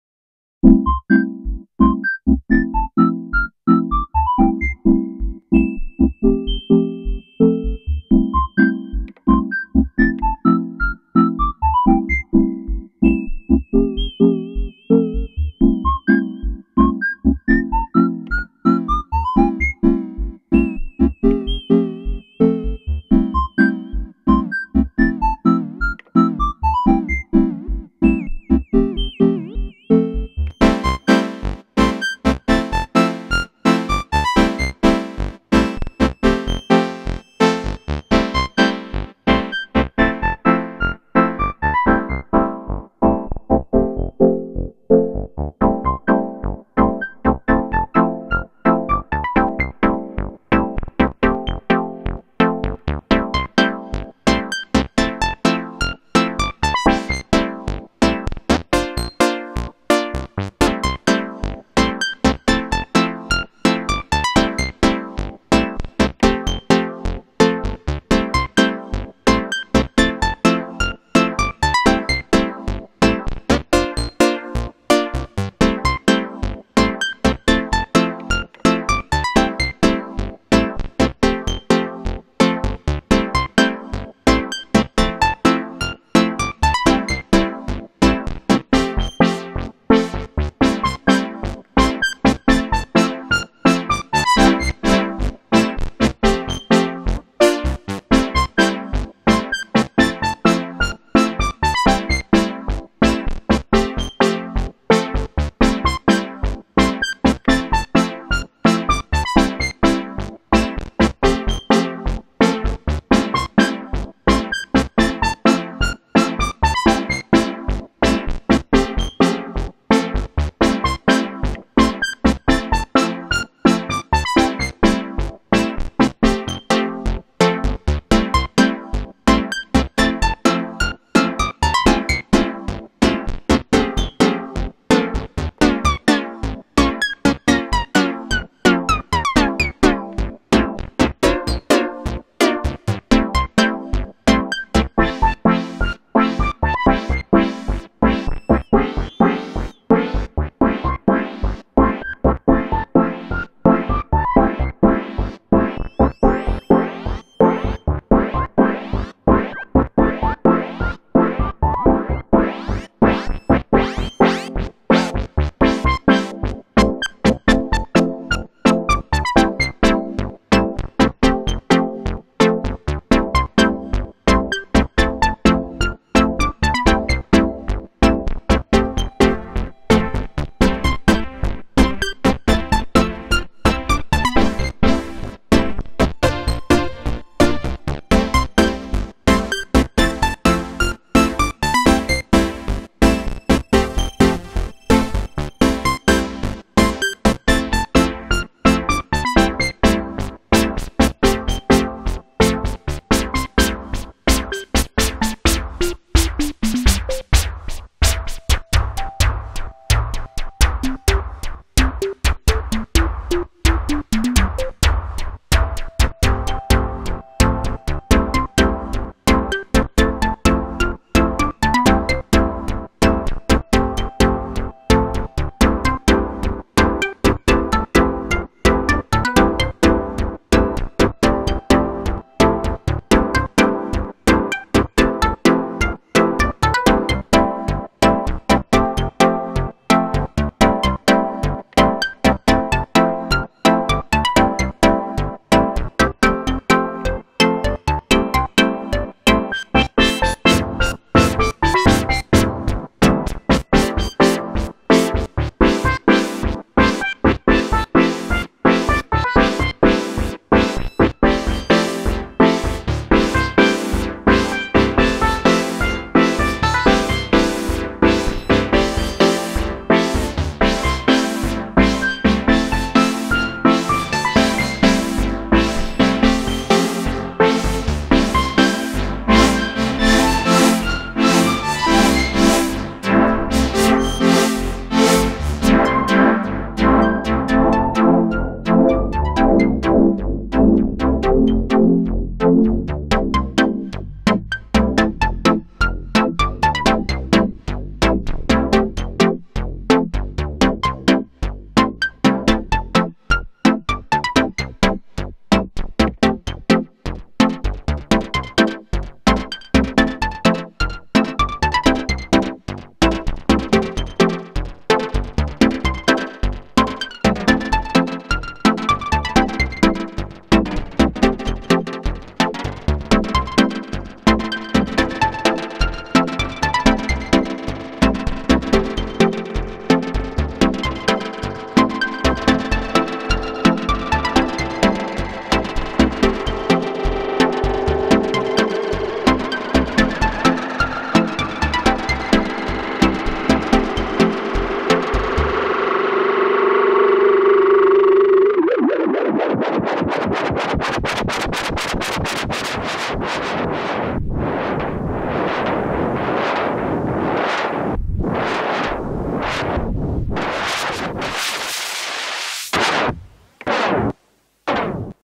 DRC Synth controled by BCR2000 [deepsounds for handluggage]
the 'more filter' version! the looped one bar sequence, including low, mid pitch and melody content played back from logic pro via behringers midi-in. to one instance of DRC! only DRCs own reverb, delay and chorus is used!
to lower the dynamic i used waves max volume postrecording!
BCR2kOverlay: made by Taktility, overlaydesign by Hendrik Gundlach